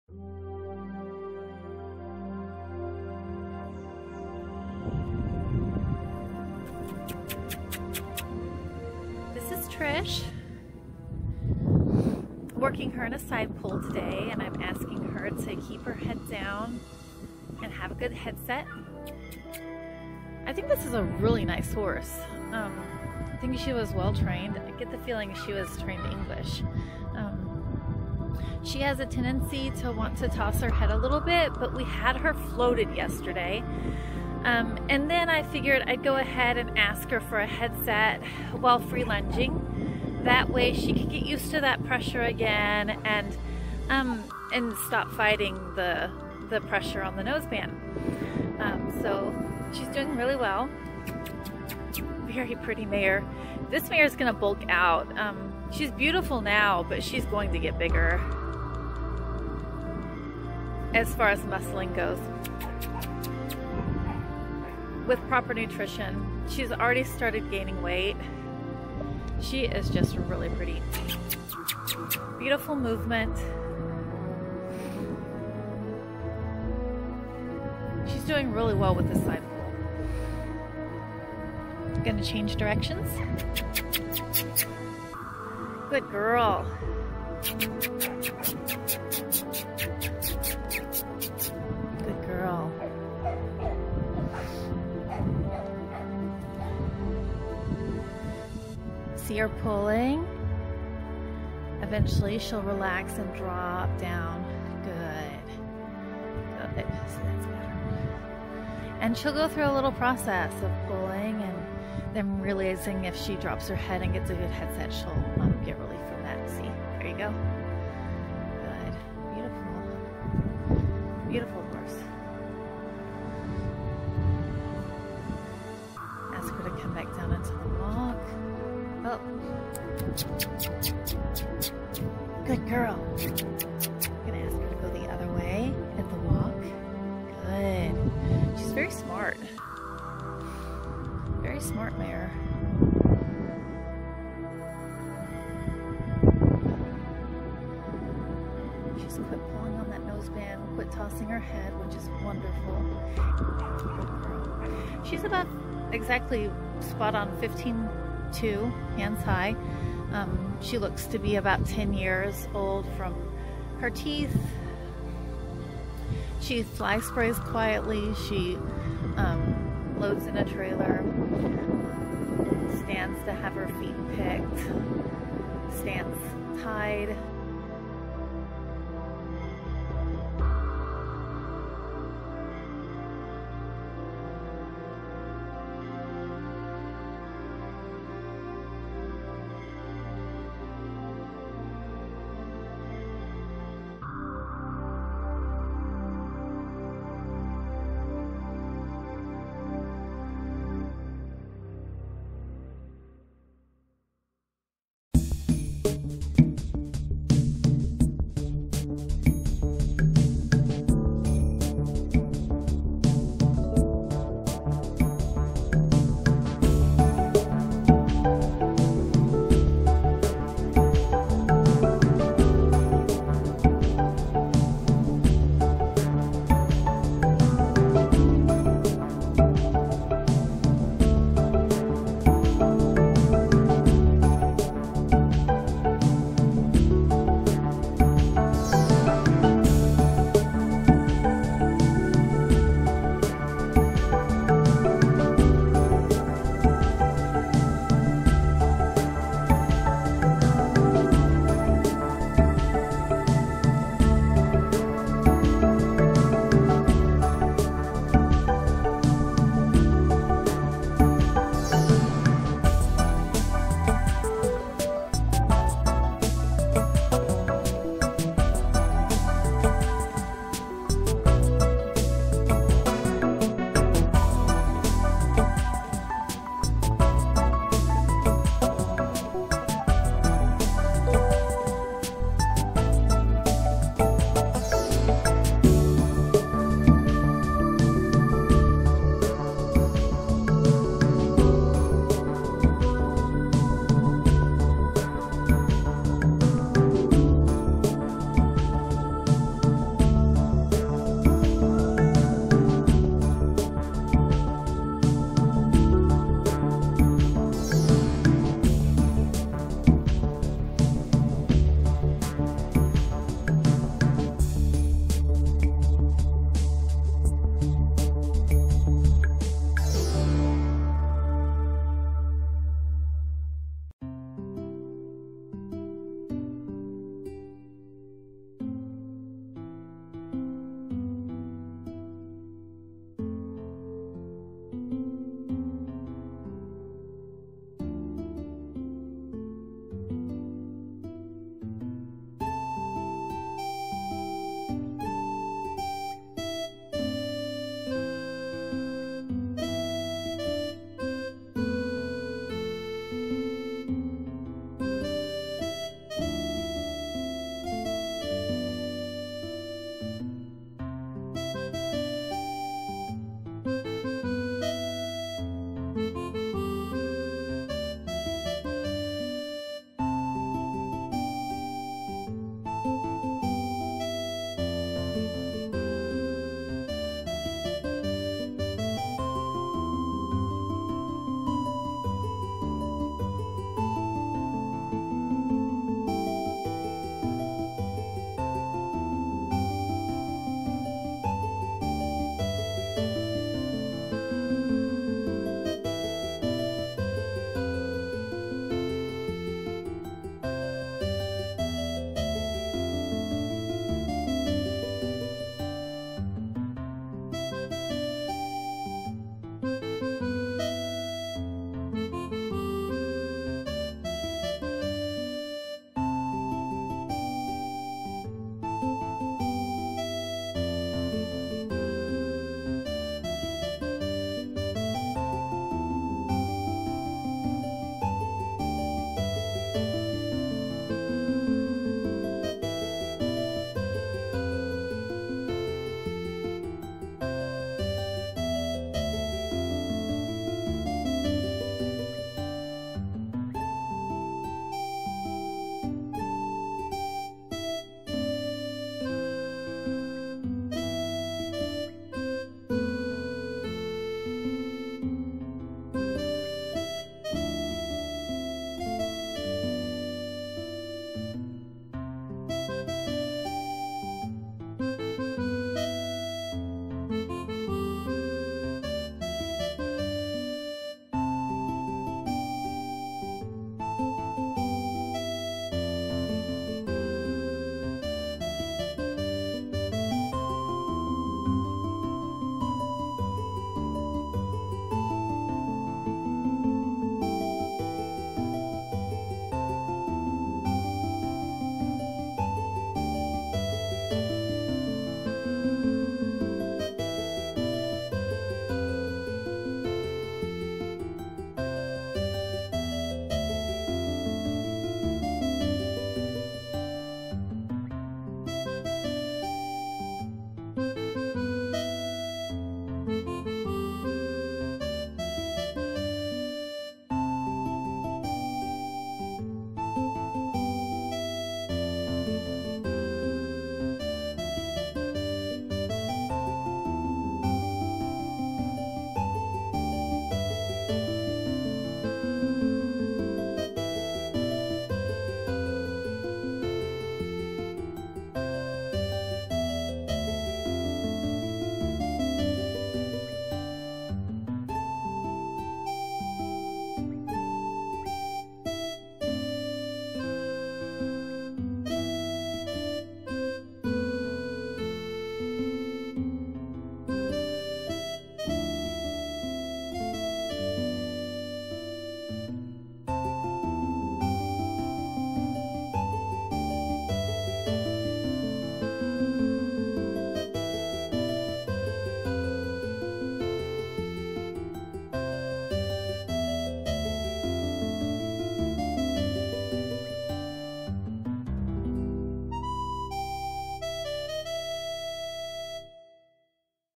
TRISH 4 2 2020
Trish
15.2HH
10 years old
Fleabitten gray mare

Broke to ride, currently getting a refresher under saddle.

Fresh coggins pulled, teeth floated.